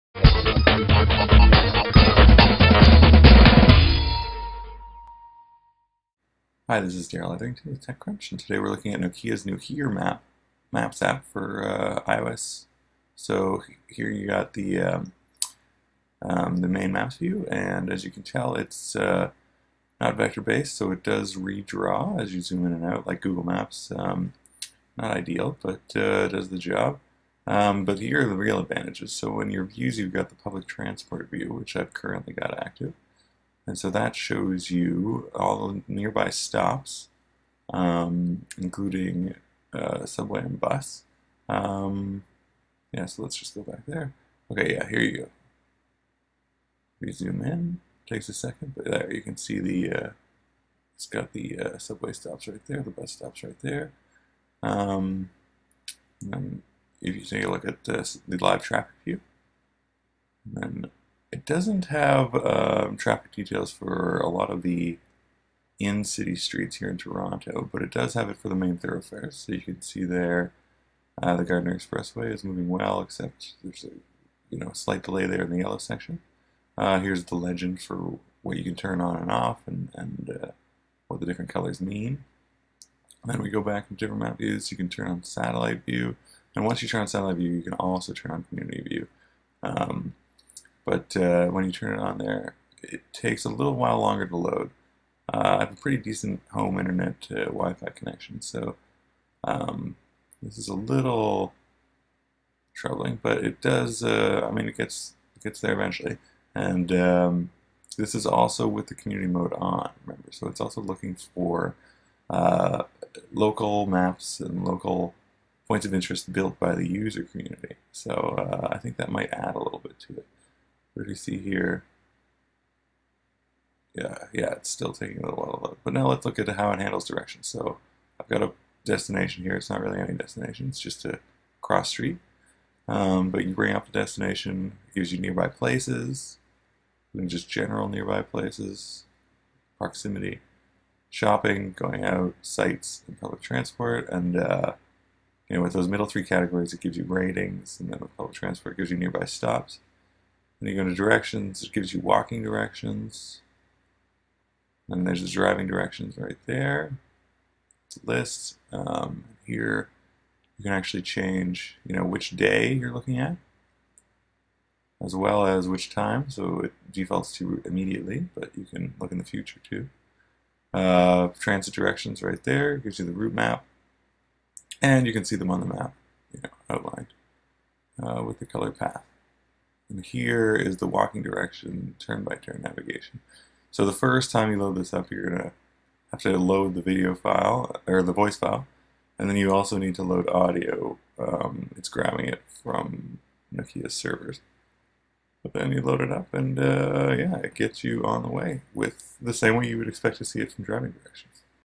Nokia Here iPhone Demo
A walkthrough of the Nokia Here maps app for iOS, with a walkthrough of some of the features that could make it a preferable alternative to Apple's own maps software.